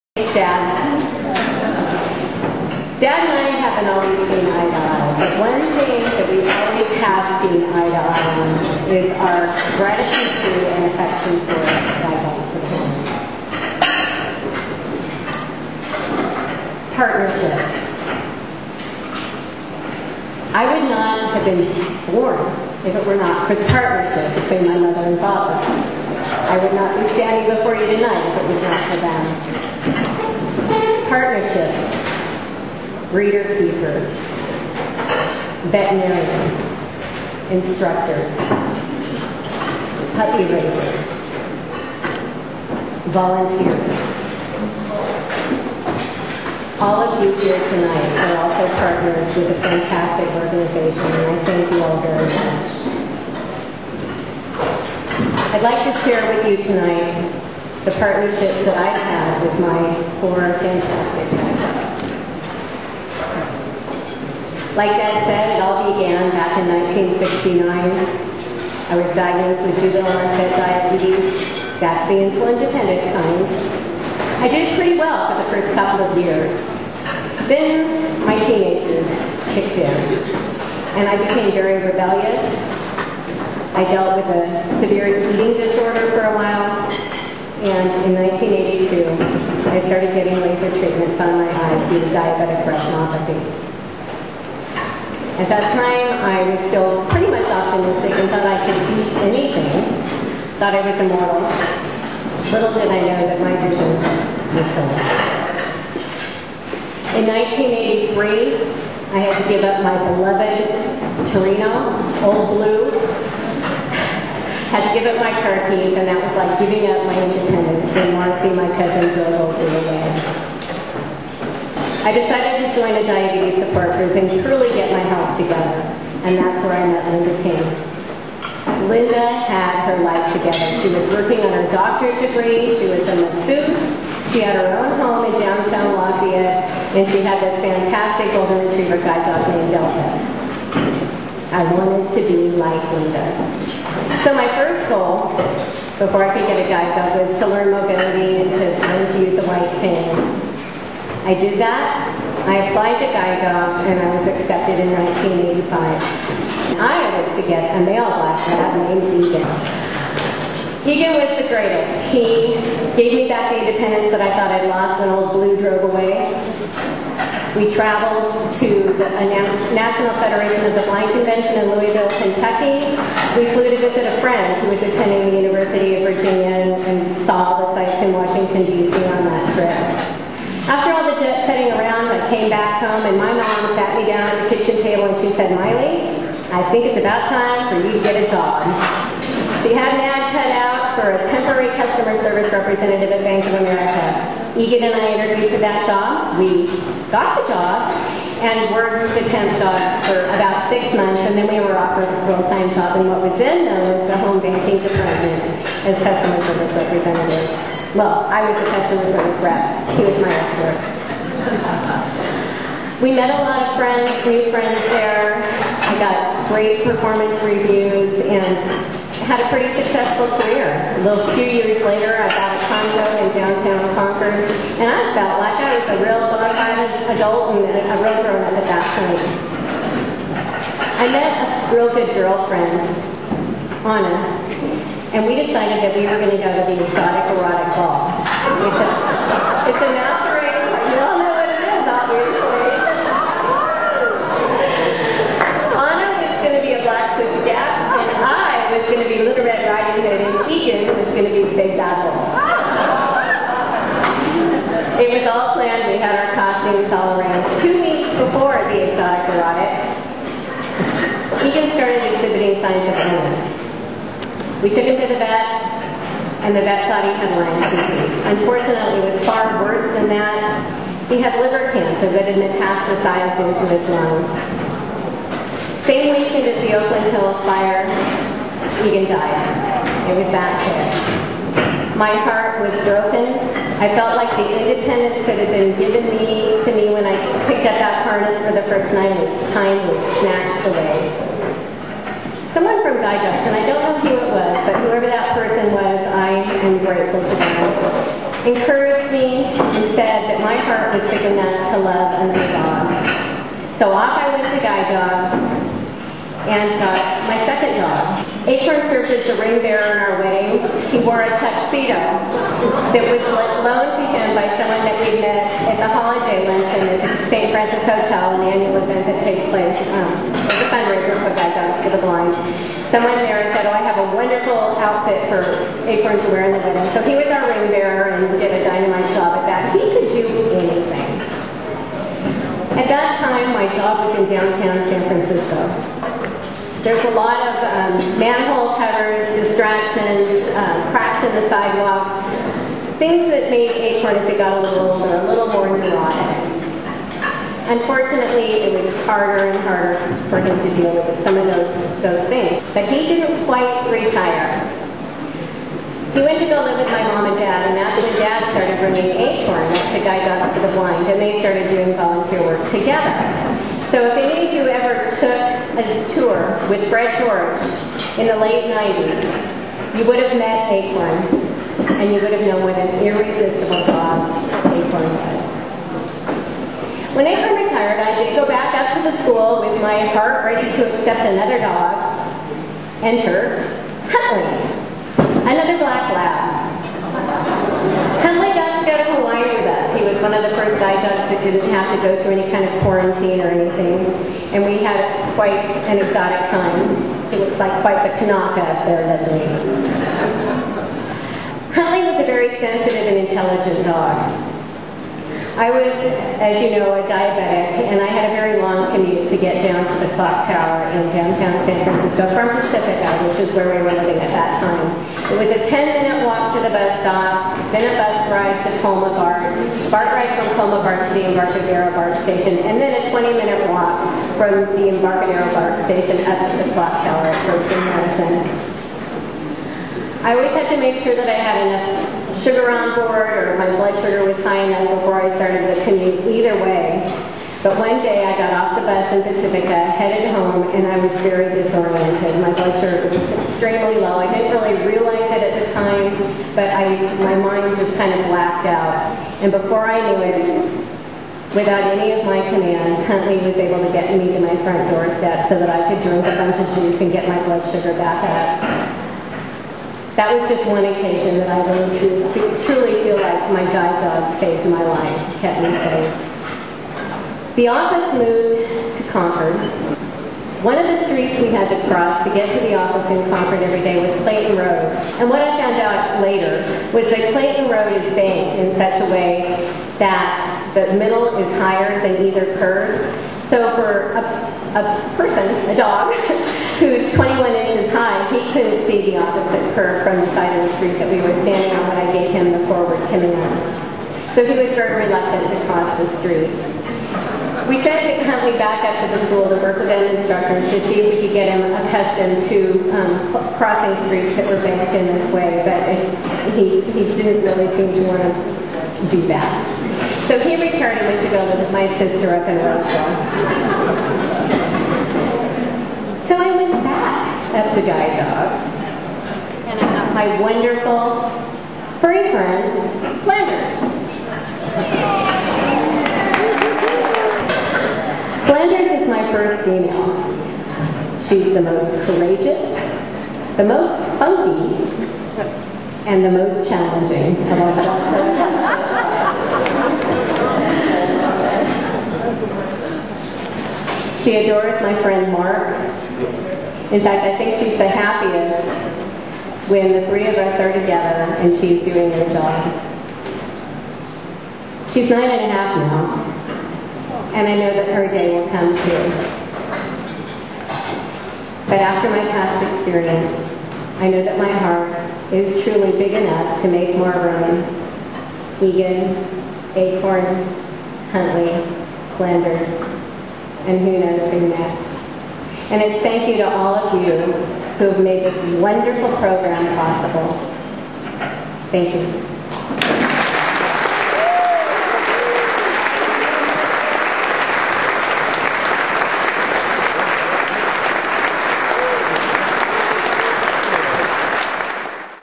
Maile George speaks at GDB's annual Wine Gala Fundraiser
GDB grad Maile with Guide Dog Flanders speaks at GDB's 7th annual Wine Gala in Napa Valley